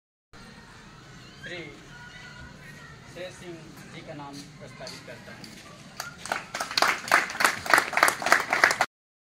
Independence Day celebrations at remote village Chhiteshgar. protection with CAPF
Independence Day at remote village Chhiteshgar. protection with CAPF